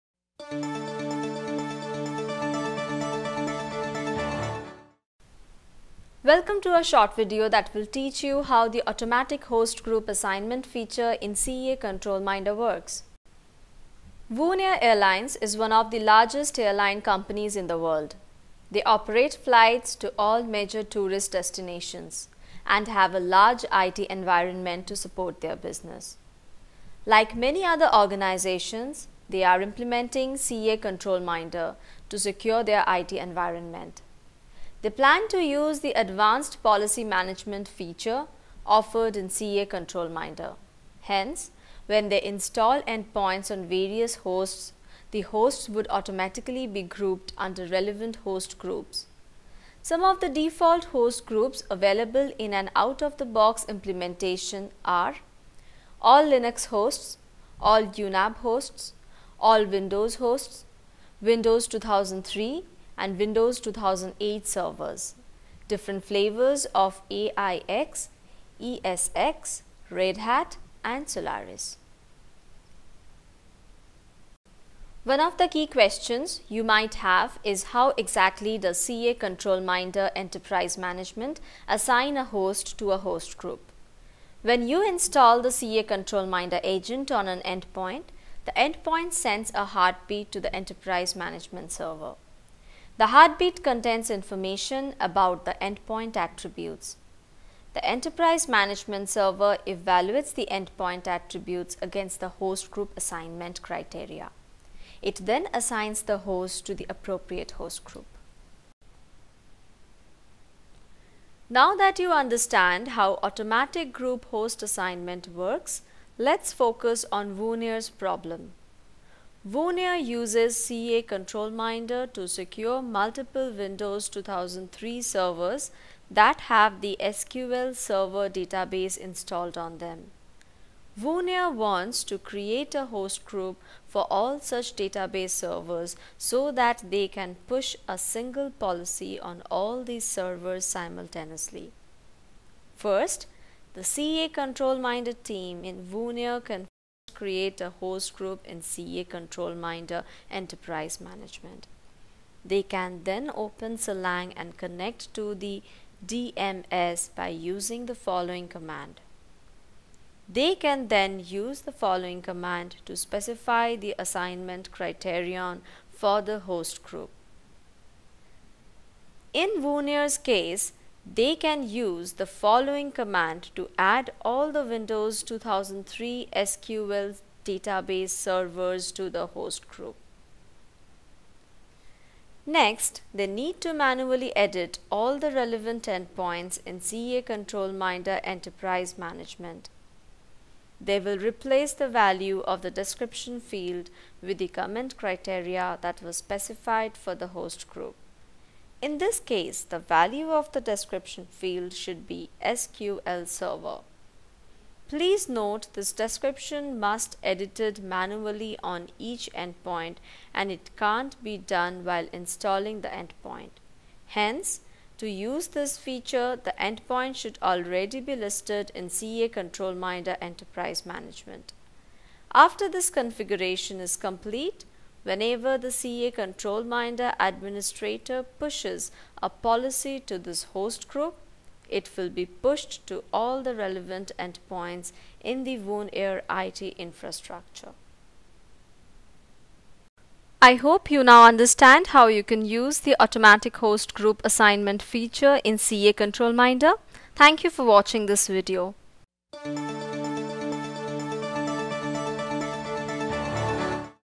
CA Privileged Identity Manager (formerly CA ControlMinder) Host Group Assignment
This short video explains how an automatic host group assignment feature in CA Privileged Identity Manager (formerly CA ControlMinder) works.